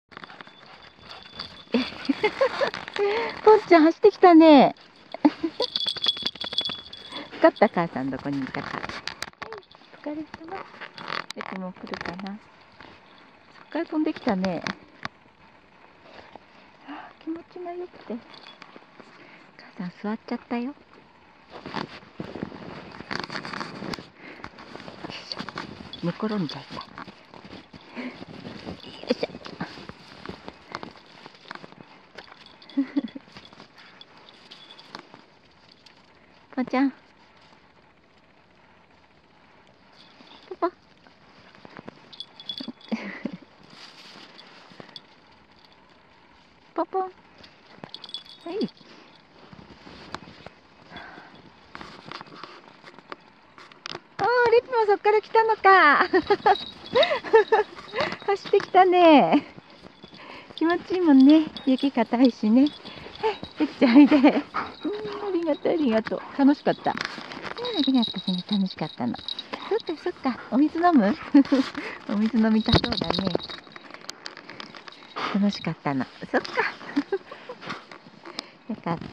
サルーキ親子！丘の上からジャジャジャジャ～ン♪（Saluki）
れぴ☆ぽぽを待っていると
丘の上からジャジャジャジャーーン＾＾♪